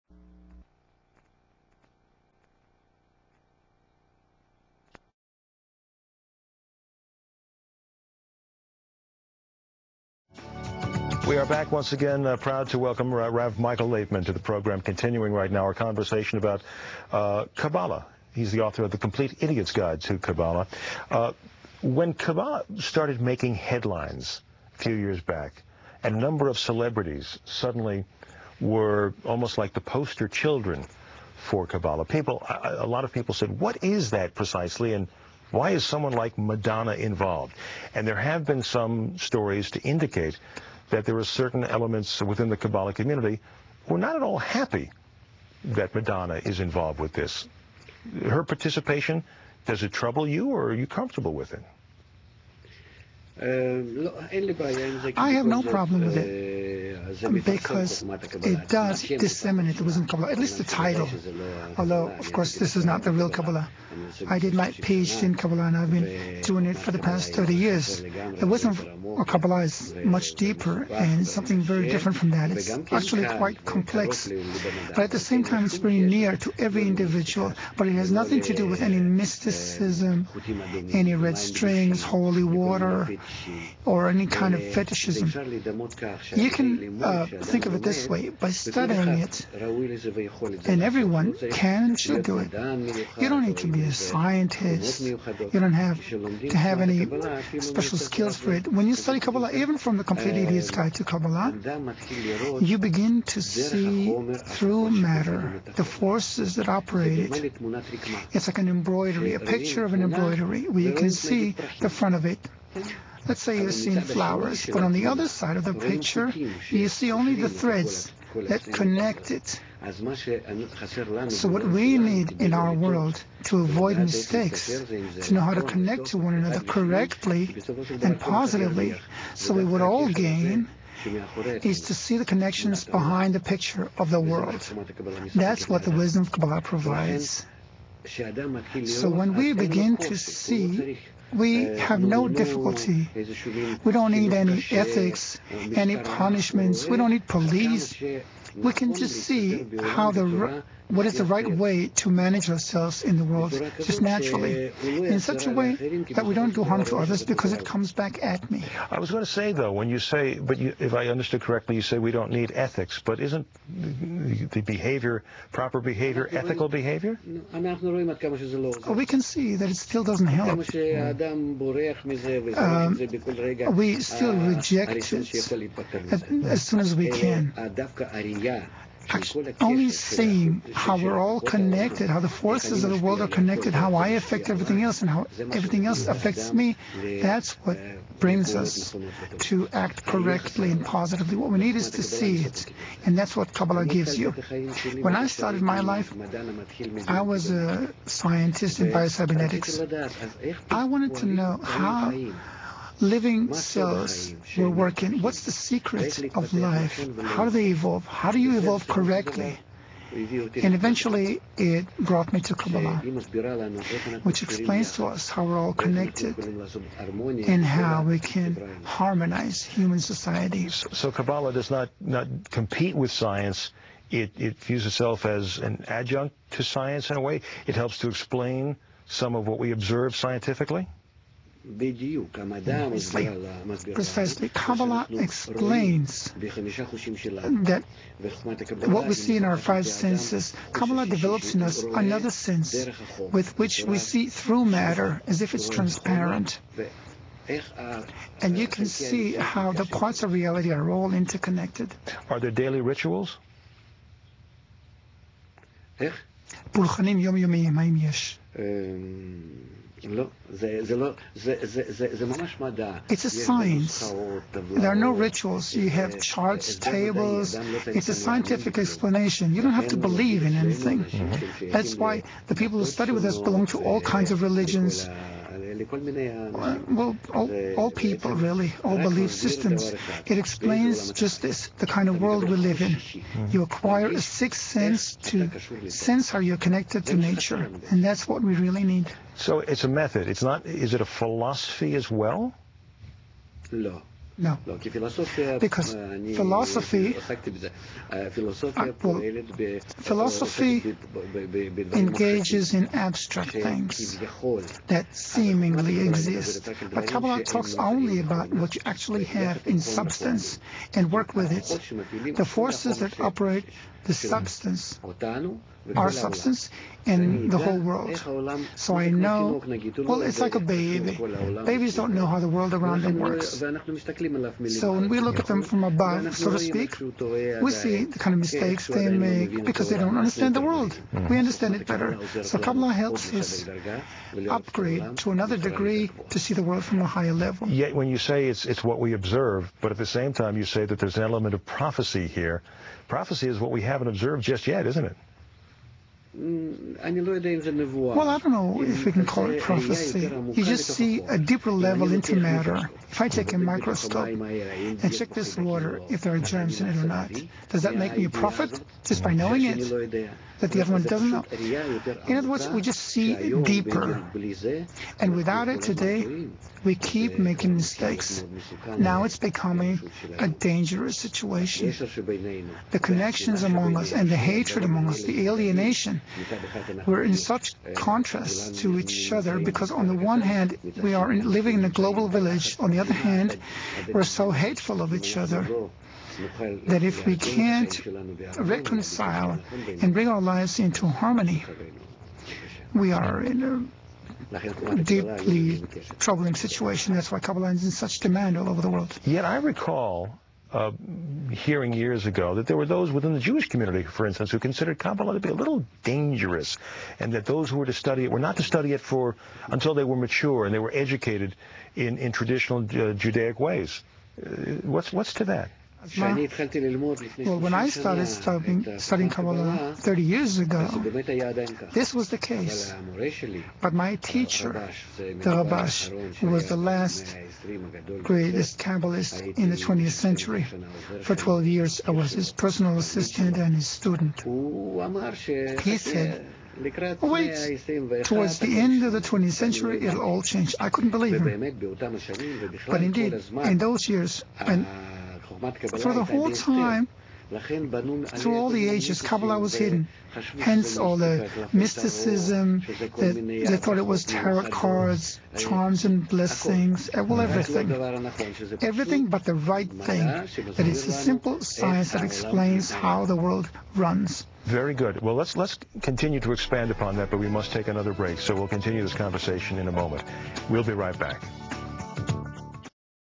Interview with Kabbalist Rav Michael Laitman on Bloomberg TV - 2007- 10 - 24 - part 2.
Rav Michael Laitman answering the questions concerning the wisdom of Kabbalah on Bloomberg TV